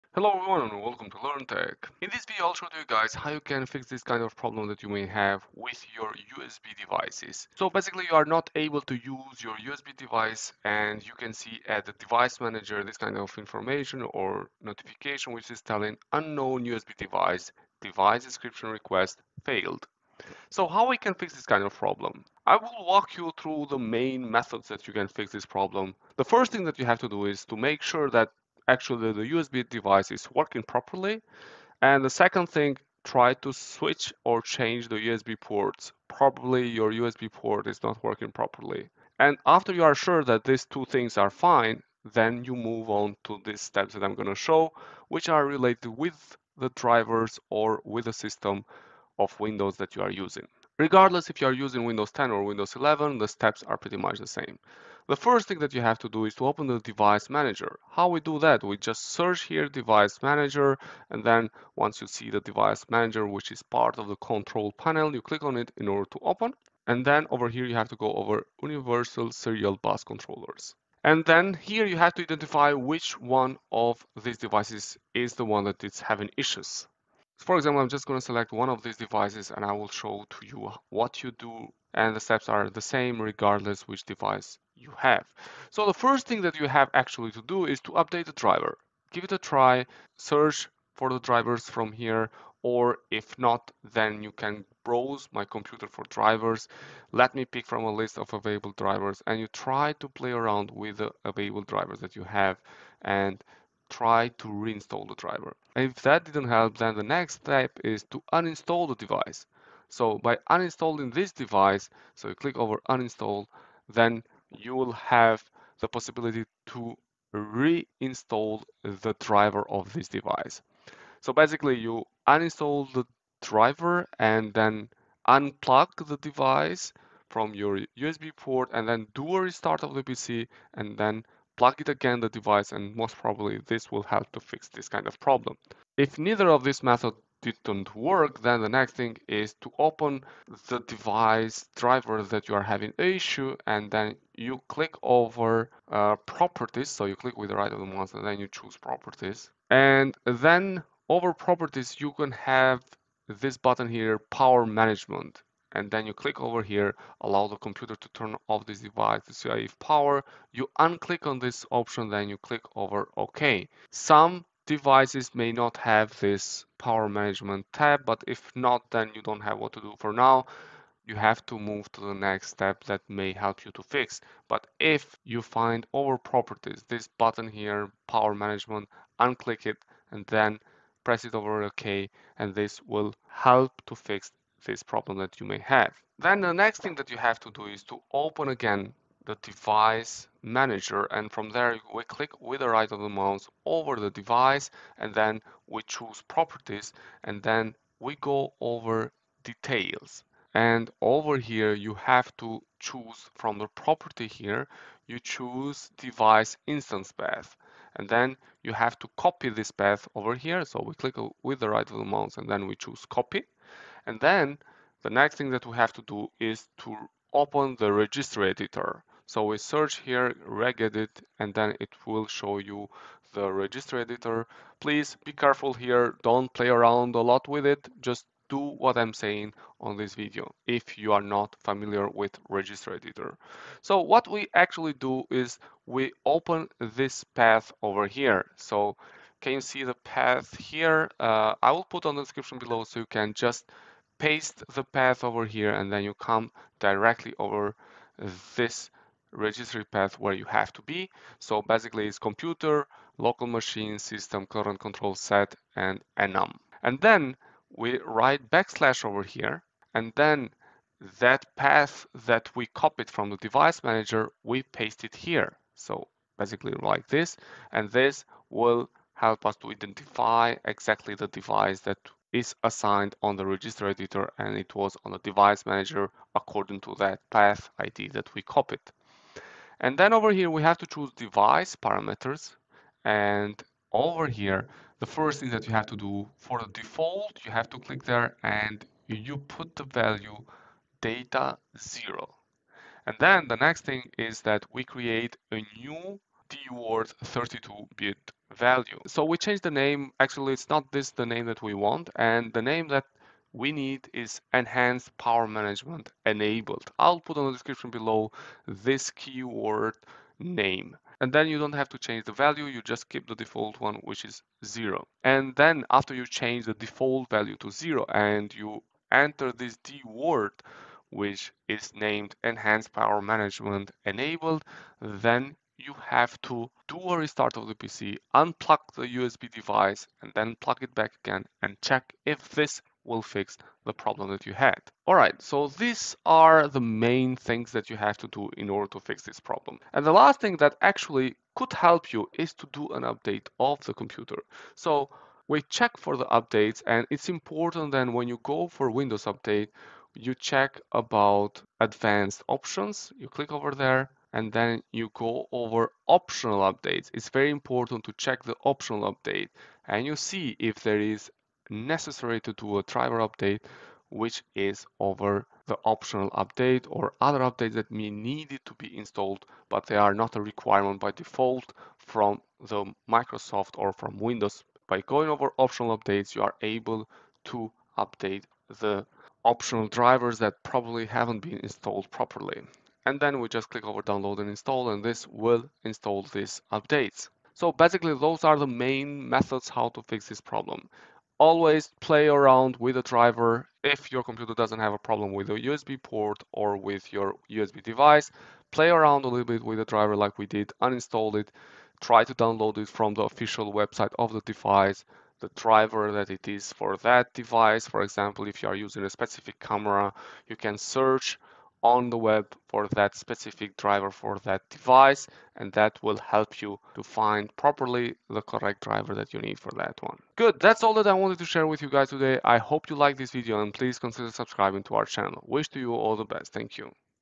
Unknown USB Device (Device Descriptor Request Failed) | Unknown USB Device Windows 11/10
Regedit path: Computer\HKEY_LOCAL_MACHINE\SYSTEM\CurrentControlSet\Enum\
DWORD32 Bit: EnhancedPowerManagementEnabled Value: 0

ALL POSSIBLE SOLUTIONS

UNKNOWN USB DEVICE (DEVICE DESCRIPTOR REQUEST FAILED)